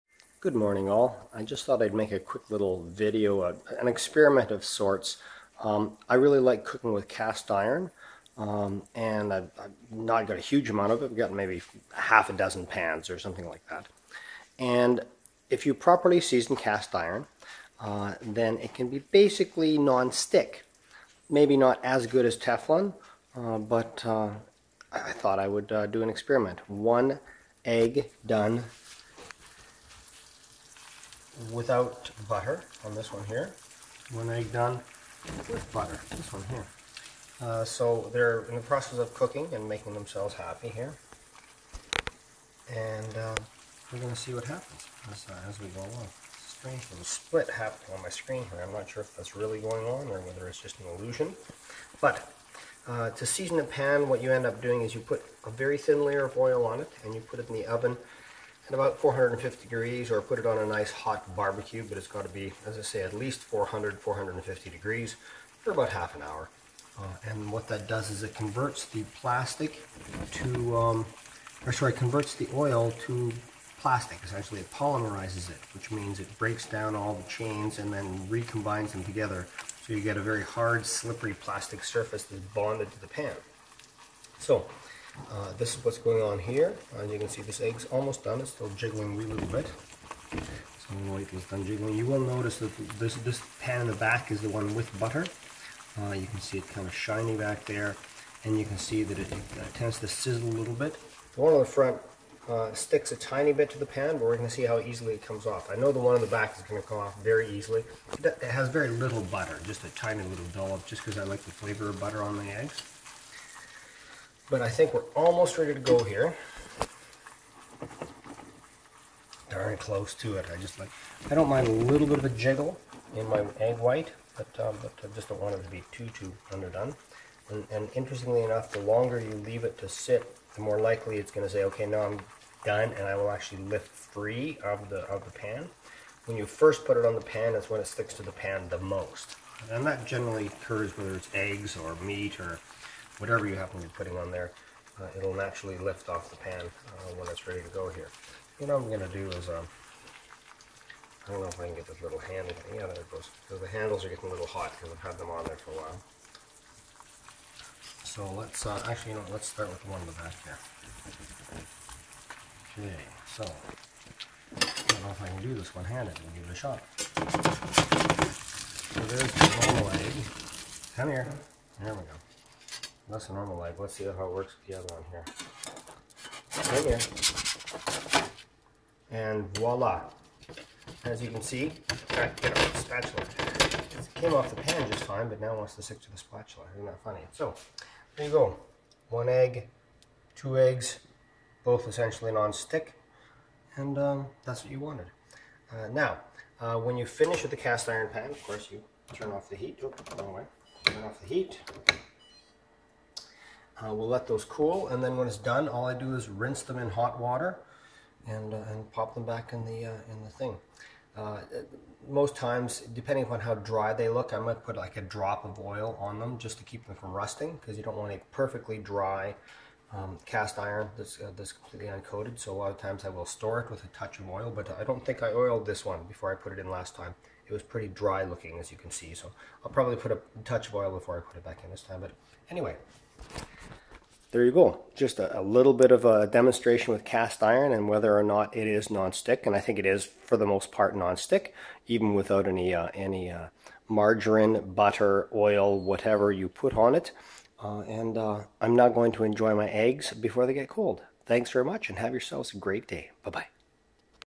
Non Stick Cast Iron
An experiment with two eggs and two cast iron pans, to gauge whether a properly seasoned pan is actually non-stick.

(Sorry for the split screen glitch.)